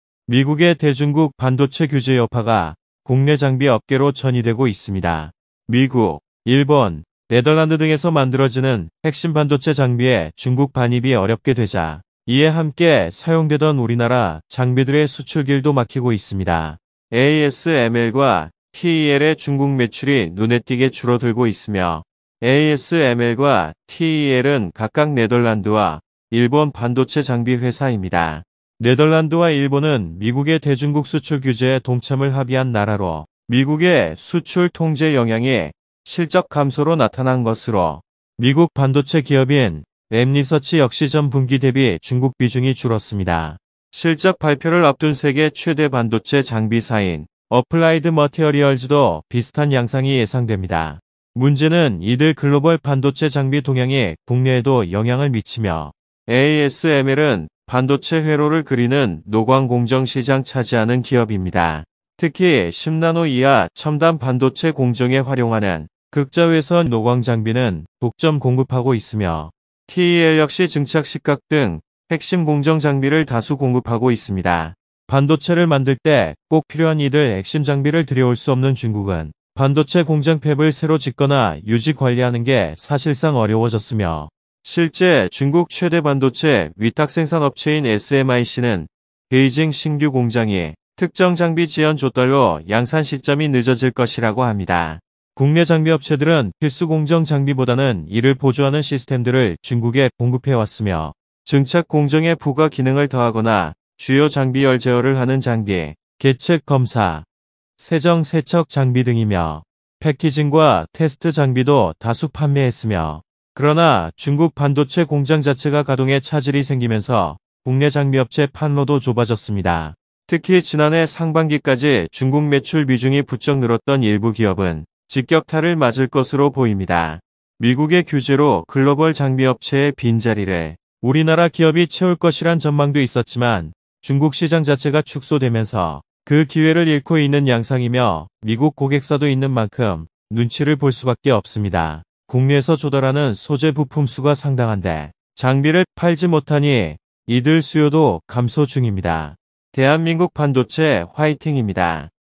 대중국반도체 제제에 따른 장비업체의 희비
미국의 대중국 반도체 규제 여파가 국내 장비 업계로 전이되고 있습니다. 미국, 일본, 네덜란드 등에서 만들어지는 핵심 반도체 장비의 중국 반입이 어렵게 되자 이에 함께 사용되던 우리나라 장비들의 수출길도 막히고 있습니다.
A S ML과 T E L의 중국 매출이 눈에 띄게 줄어들고 있으며

. 4분기 ASML의 중국 매출 비중은 전체(약 6조5000억원)의 약 9%로, 전년 동기 대비 13%포인트(P) 감소했다. TEL의 4분기 중국 반도체 장비 매출 비중도 전년 동기 대비 4.7%P 감소한 22.4%로 나타났다. TEL 4분기 전체 반도체 장비 실적은 약 4조3900억원이었다.

A S ML과 T E L은 각각 네덜란드와 일본 반도체 장비 회사입니다. 네덜란드와 일본은 미국의 대중국 수출 규제에 동참을 합의한 나라로, 미국의 수출 통제 영향이 실적 감소로 나타난 것으로. 미국 반도체 기업인 램리서치 역시 전 분기 대비 중국 비중이 줄었습니다. 실적 발표를 앞둔 세계 최대 반도체 장비사인 어플라이드머티어리얼즈도 비슷한 양상이 예상됩니다.
문제는 이들 글로벌 반도체 장비 동향이 국내에도 영향을 미치며. A S ML은 반도체 회로를 그리는 노광 공정 시장  차지하는 기업입니다. 특히 10나노 이하 첨단 반도체 공정에 활용하는 극자외선(EUV) 노광 장비는 독점 공급하고 있으며.  T E L 역시 증착·식각 등 핵심 공정 장비를 다수 공급하고 있습니다.

반도체를 만들 때 꼭 필요한 이들 핵심 장비를 들여올 수 없는 중국은 반도체 공장(팹)을 새로 짓거나 유지·관리하는 게 사실상 어려워졌으며. 실제 중국 최대 반도체 위탁생산 업체인 S M I C는 베이징 신규 공장이 특정 장비 지연 조달로 양산 시점이 늦어질 것이라고 합니다.

국내 장비업체들은 필수 공정 장비보다는 이를 보조하는 시스템들을 중국에 공급해왔으며. 증착 공정에 부가 기능을 더하거나 주요 장비 열 제어를 하는 장비, 계측·검사, 세정·세척 장비 등이며. 패키징과 테스트 장비도 다수 판매했으며. 그러나 중국 반도체 공장 자체가 가동에 차질이 생기면서 국내 장비업체 판로도 좁아졌습니다. 특히 지난해 상반기까지 중국 매출 비중이 부쩍 늘었던 일부 기업은 직격타를 맞을 것으로 보입니다.
미국의 규제로 글로벌 장비업체의 빈자리를 우리나라 기업이 채울 것이란 전망도 있었지만 중국 시장 자체가 축소되면서 그 기회를 잃고 있는 양상이며 미국 고객사도 있는 만큼 눈치를 볼 수 밖에 없습니다.

소재·부품 분야도 마찬가지다. 반도체 장비업체가 국내에서 조달하는 소재·부품 수가 상당한데 장비를 팔지 못하니 이들 수요도 감소 중이다.

한 반도체 소모품 업체 대표는 “미·중 갈등 여파가 작년 하반기부터 체감되기 시작했다”며 “중국이 침체될 경우 국내 소부장도 영향을 피하기 어려울 것”이라고 말했다.